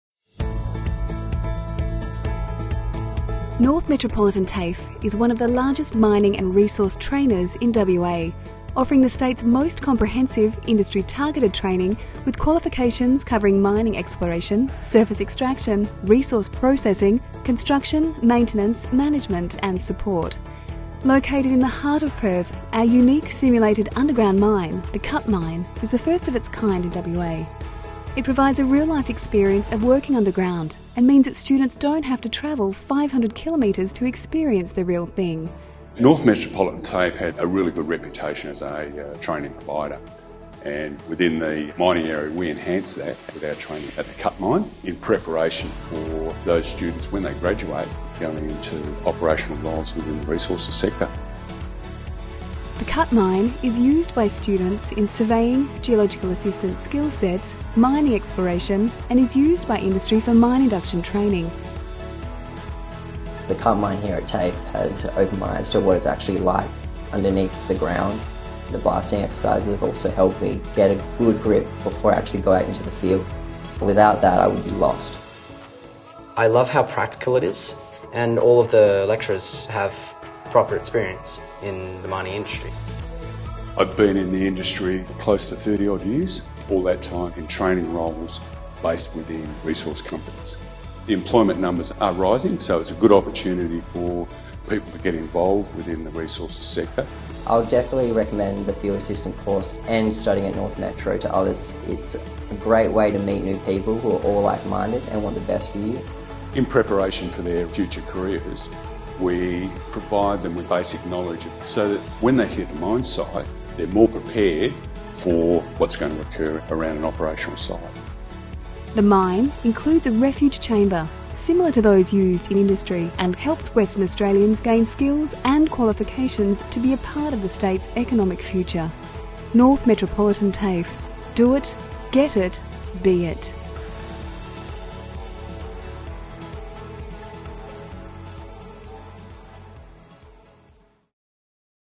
North Metropolitan TAFE's CUT Mine
North Metropolitan TAFE is one of the largest mining and resource trainers in WA. Located in the heart of Perth, our unique simulated underground mine, the CUT Mine, is the first of its kind in WA. The mine provides students with real-life experience of working underground.